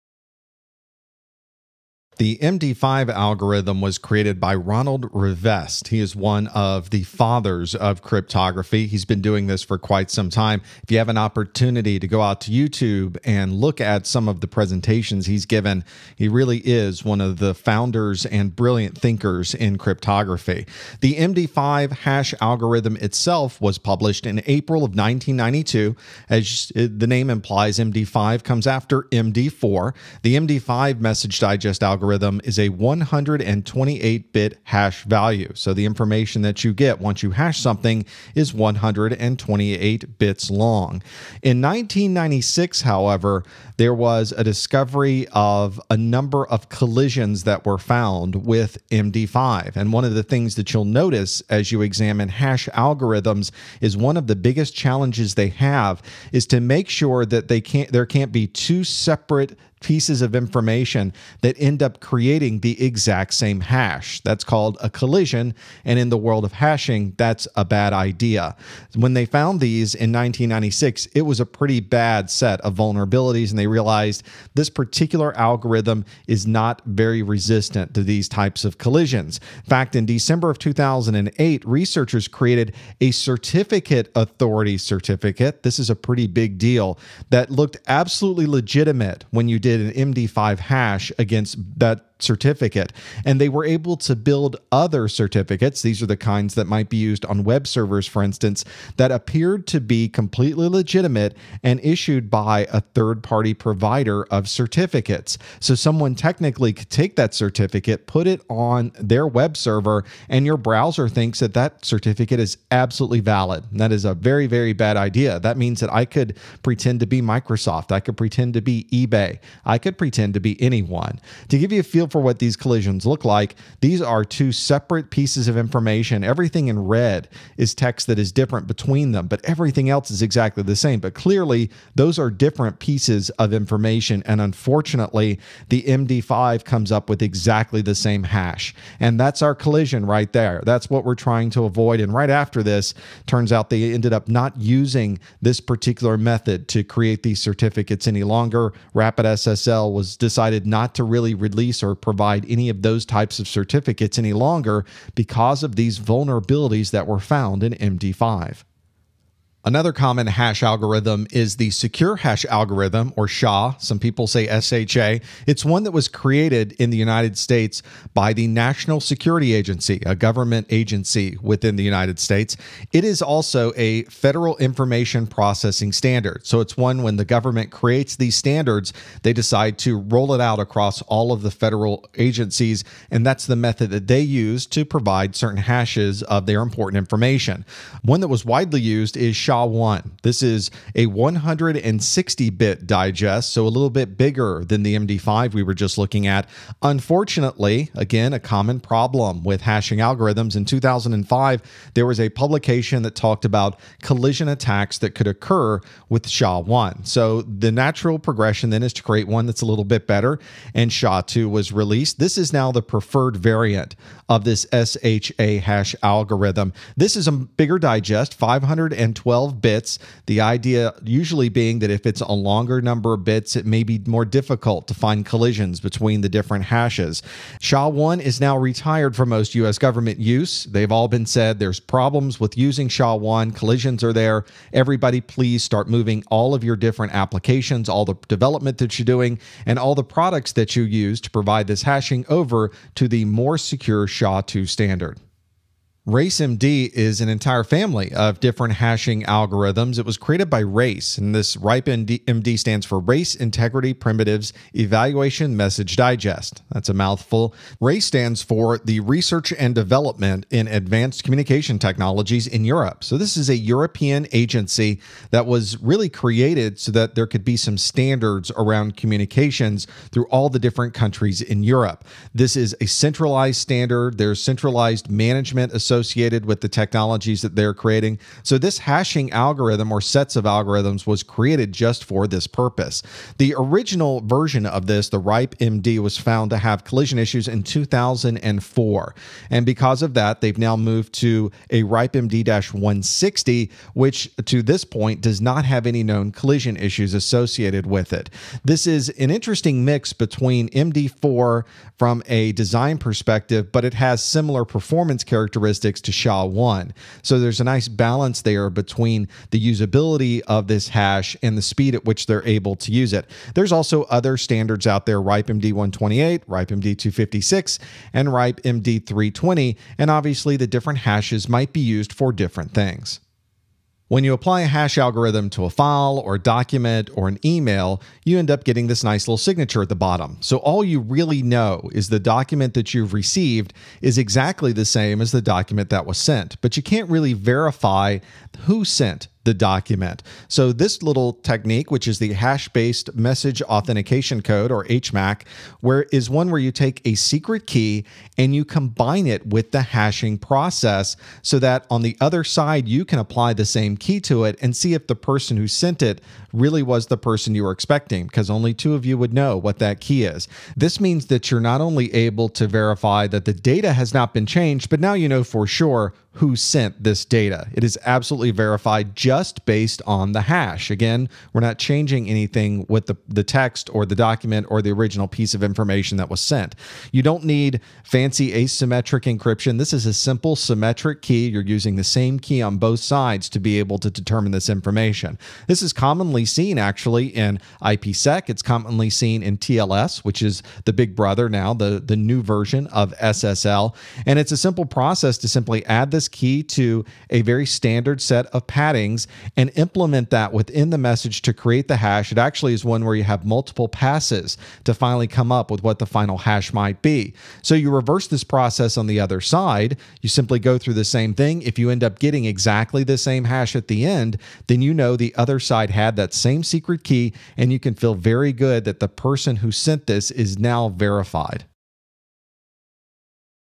Cryptographic Hash Functions - CompTIA Security+ SY0-401: 6.2
There are many hash functions for many different circumstances. In this video, you’ll learn about MD5, SHA, RIPEMD, and HMAC hash functions.